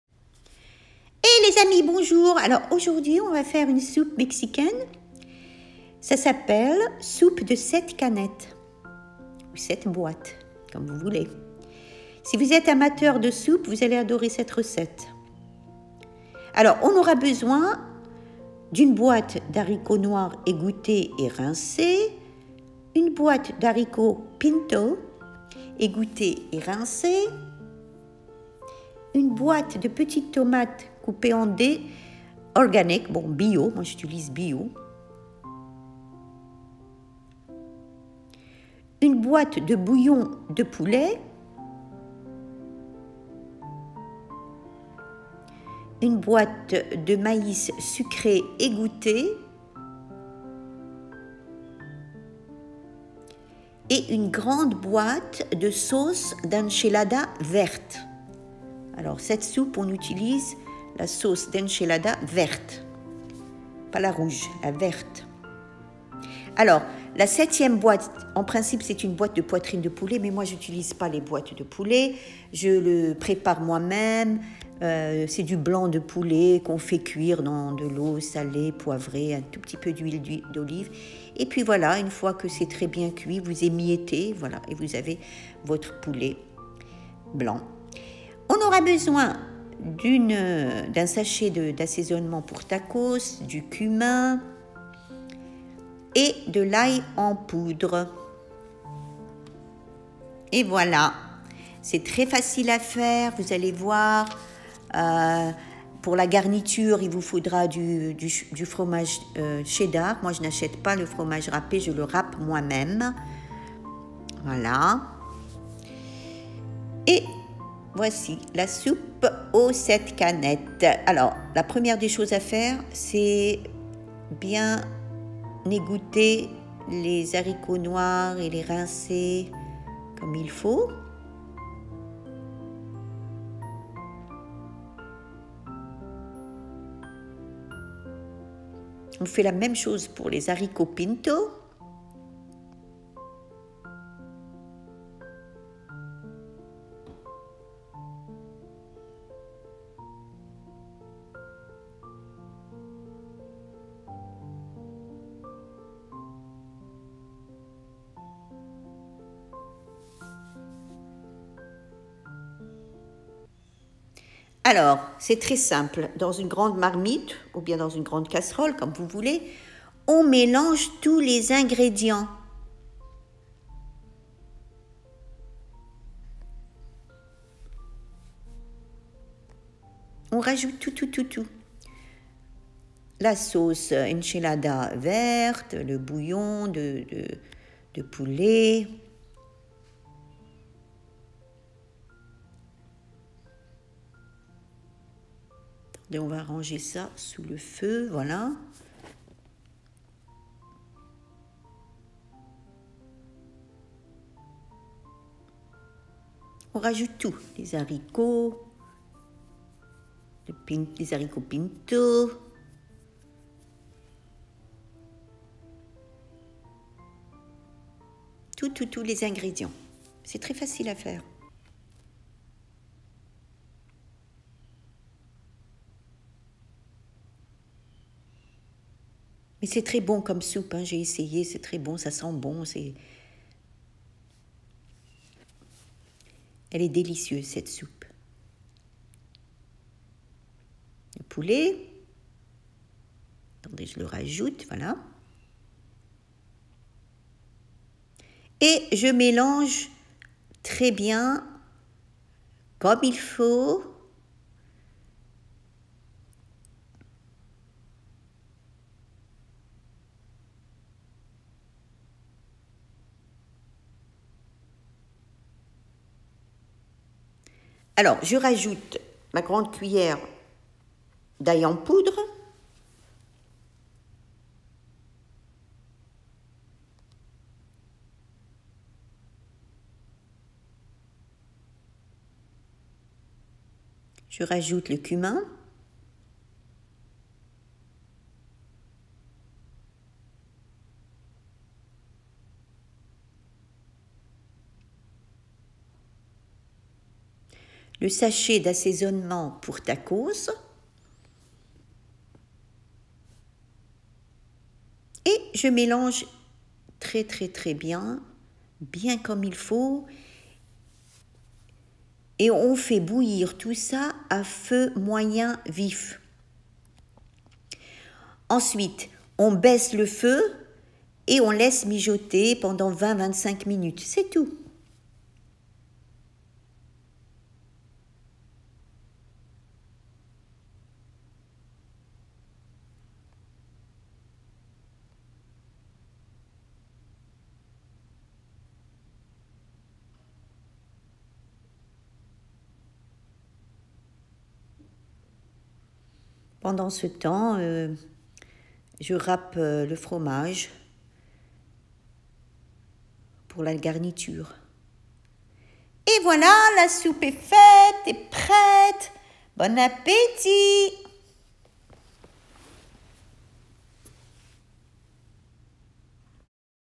SOUPE DE 7 CANETTES
Ingrédients :
1 boite d’haricots noirs égouttés et rinçés
1boîte d’haricots pintos égouttés et rinçés
1 boîte de petites tomates en dés
1boîte de bouillon de poulet
1boîte de maïs sucré, égoutté
1grande boîte de sauce d’enchillada Verte
1 boîte de poitrine de poulet égouttée
Pour l’assaisonnement :
1 sachet d’assaisonnement pour Tacos
1 cuillère à café de cumin
1 cuillère à café d’ail en poudre
Garniture : fromage râpé